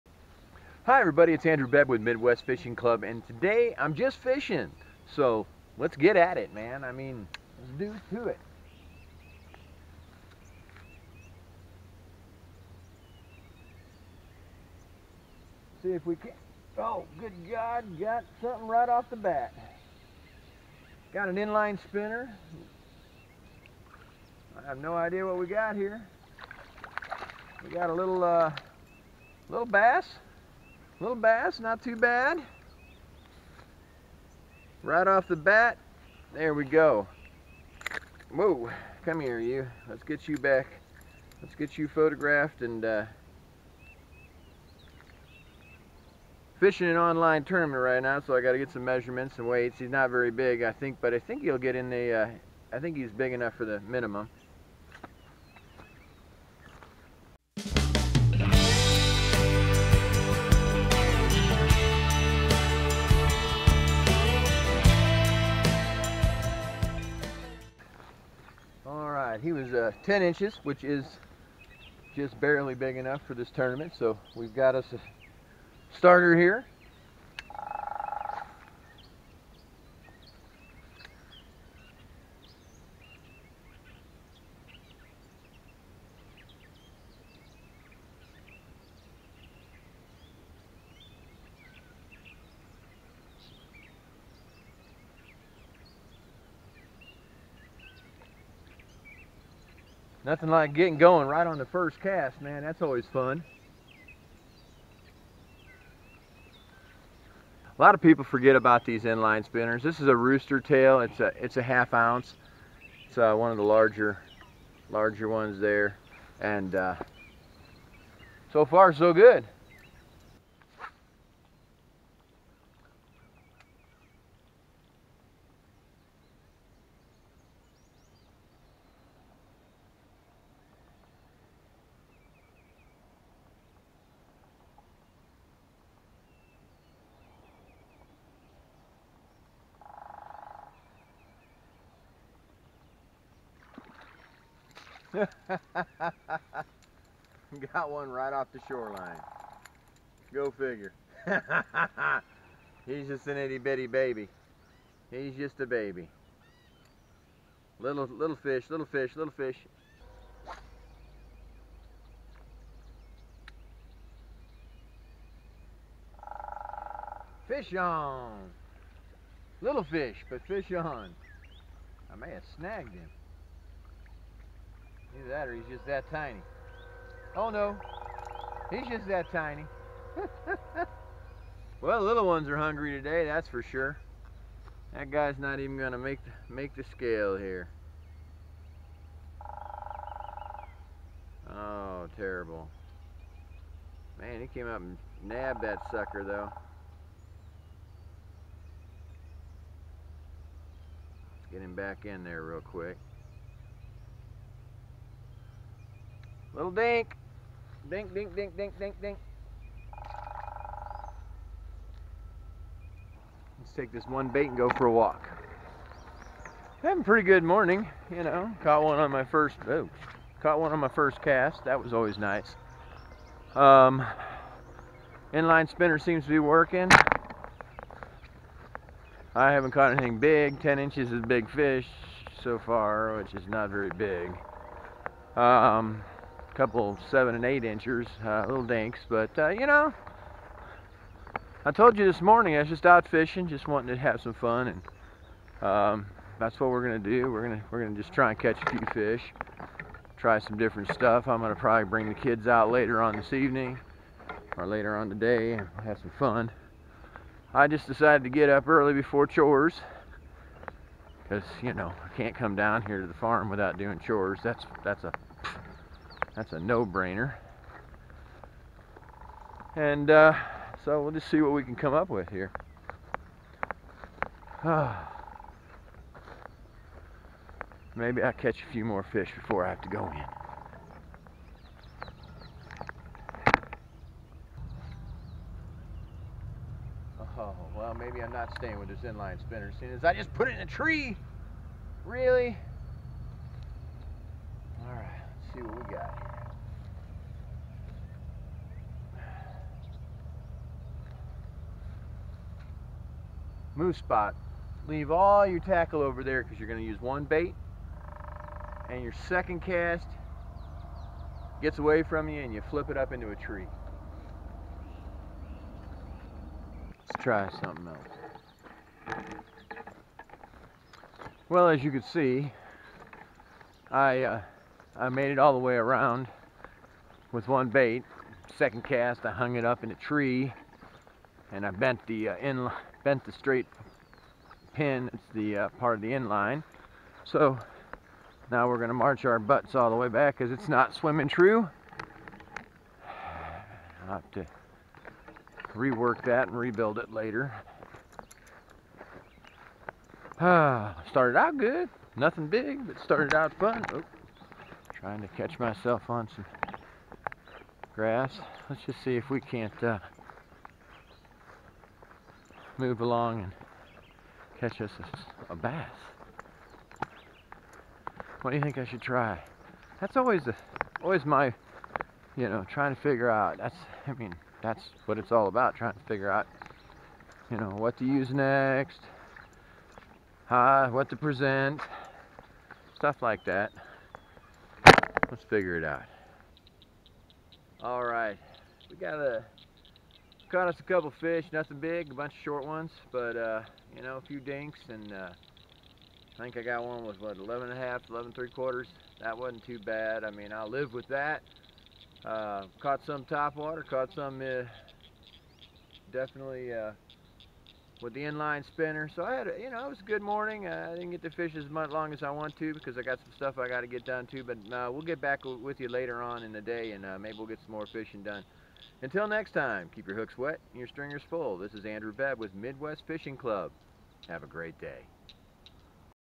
Pond Bass Fishing with a Rooster Tail Inline Spinner
Pond Bass Fishing with a Rooster Tail Inline Spinner is what you get. I have to admit that these are not the largest of bass but sometimes the big girls do not want to play.

Fishstix Rod Company
Fishstix Rod Co
This video property of Midwest Fishing Club and Andrew Bebb